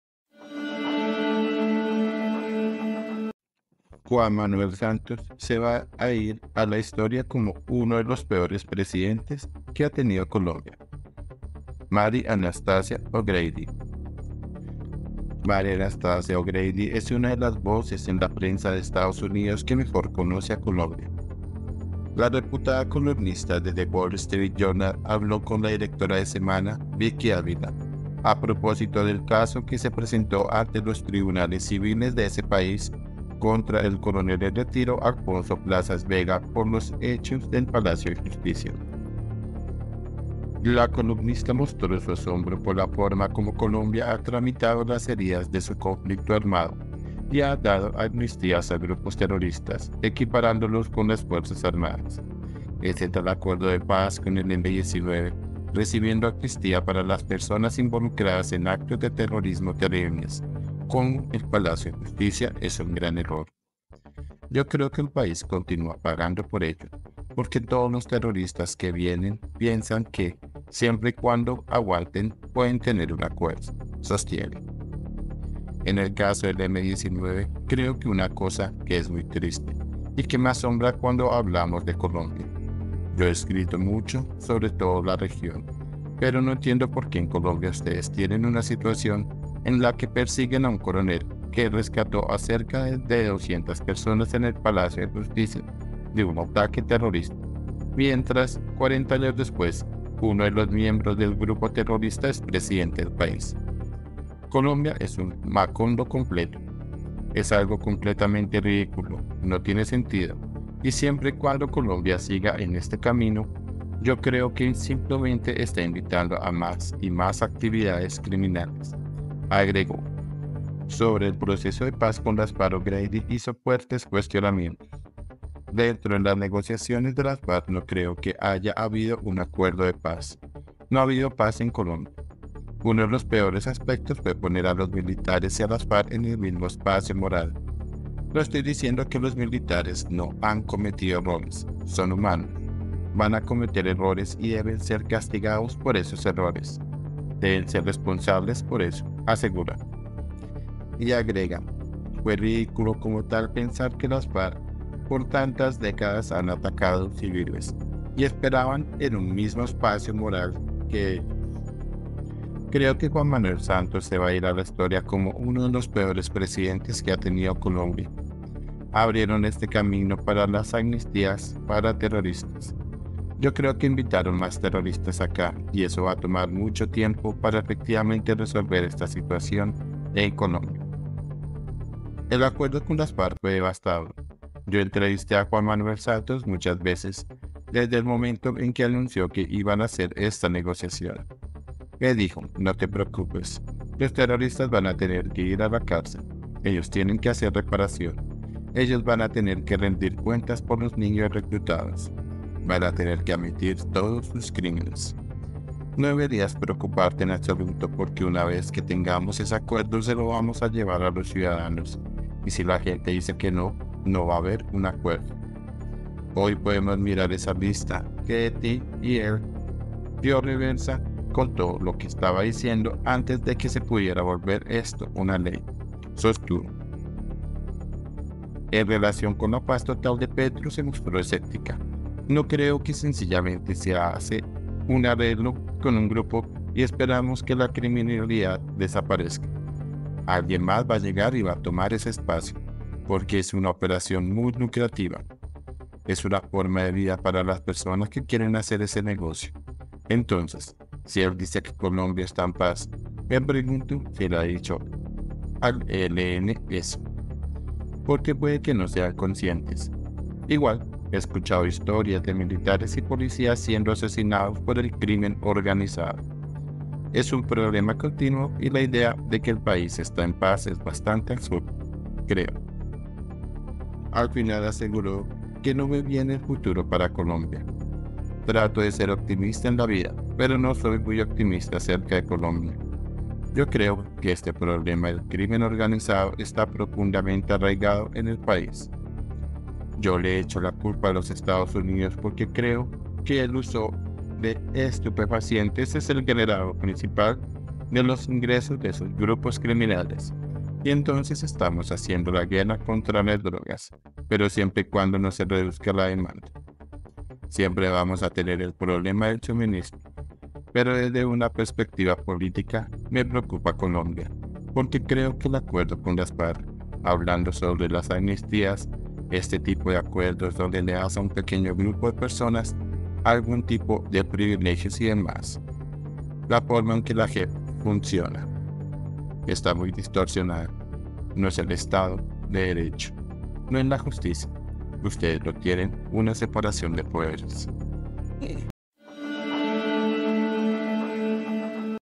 ❌ "SANTOS SE VA A IR A LA HISTORIA COMO UNO DE LOS PEORES PRESIDENTES QUE HA TENIDO COLOMBIA"
Mary Anastasia O’Grady es una de las voces en la prensa de Estados Unidos que mejor conoce a Colombia. La reputada columnista de The Wall Street Journal habló con la directora de SEMANA, Vicky Dávila, a propósito del caso que se presentó ante los tribunales civiles de ese país contra el coronel (r) Alfonso Plazas Vegas por los hechos de Palacio Justicia.
SEMANA